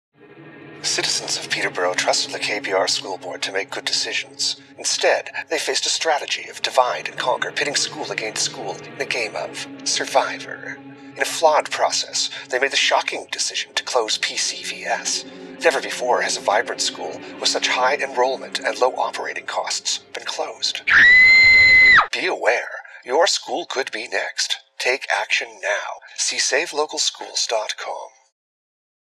Save Local Schools 2nd PCVS TV commercial
In a decision that could only have been made in the Twilight Zone the KPR school board decided to close PCVS. This nightmare is playing itself out throughout many communities across Ontario, with flawed ARC processes and school boards that are unaccountable. See savelocalschools.com